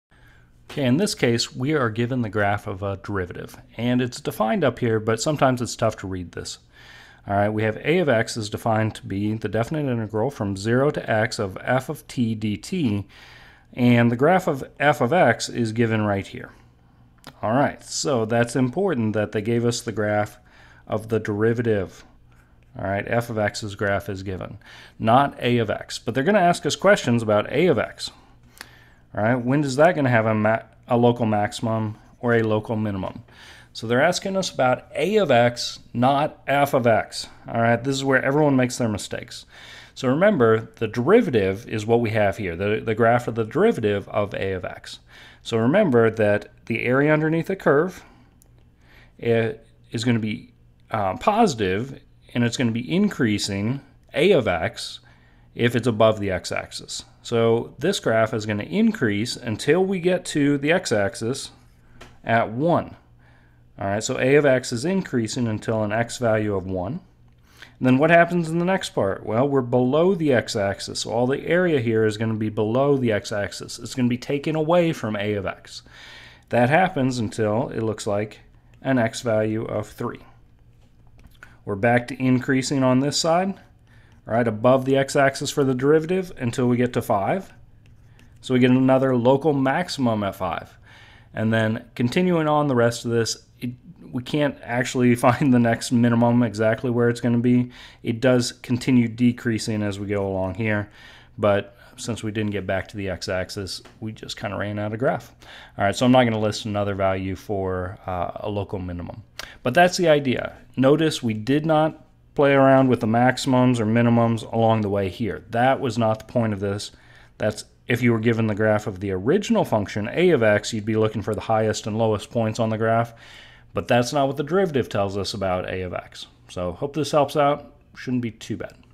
Local Max/Min | Definite Integral | Derivative Graph
In this example, we define a function as a definite integral and are given the graph of its derivative.  We then want to locate the values of x where the function will have local maximum and minimum values.